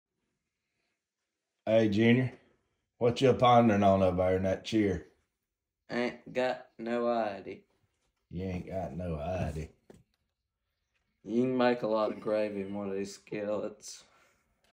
Jr had an epiphany…and a skillet
Mrs Lisa belts out a beauty on the front porch of a rustic cabin in Summerville, Georgia.